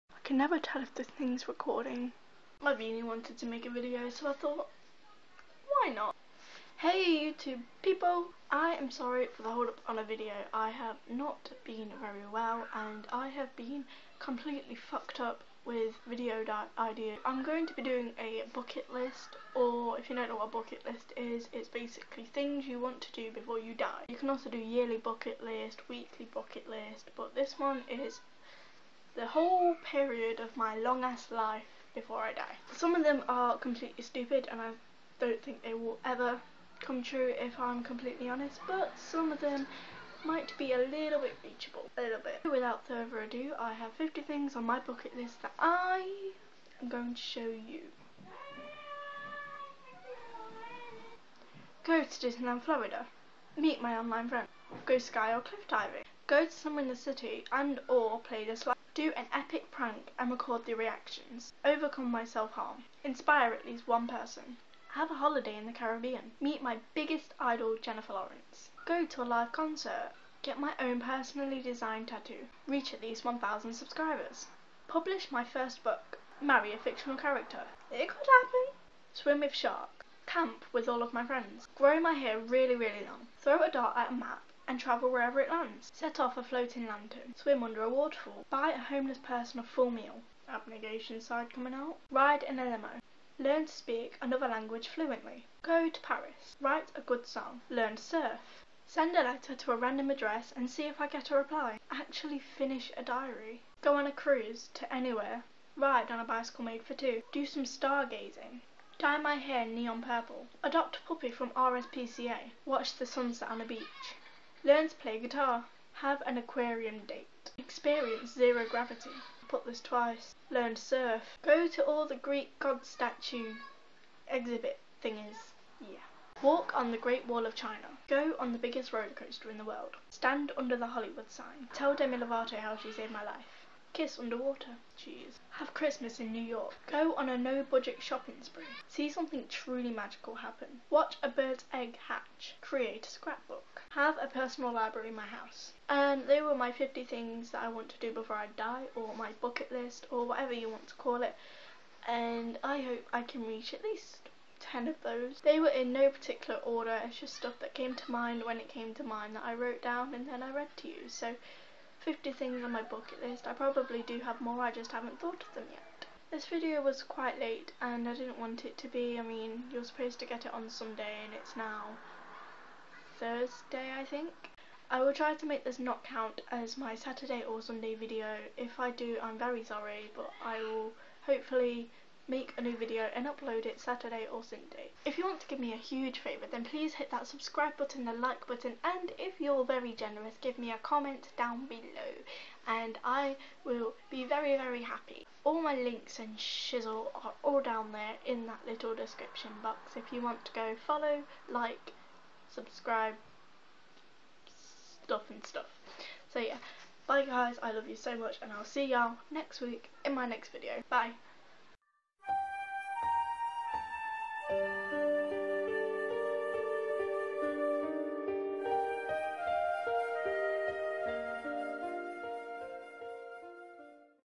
Bucket List - Before I Die - RandomRainbow
HEY! Here's a bucket list of things I want to do before I die :3
There are many more, hehe.
So, it's pretty straight forward.
What do you want to do before you die? Let me know!
What video do you want me to do? Comment to tell me!

Hope you liked it. LOVE YA;xx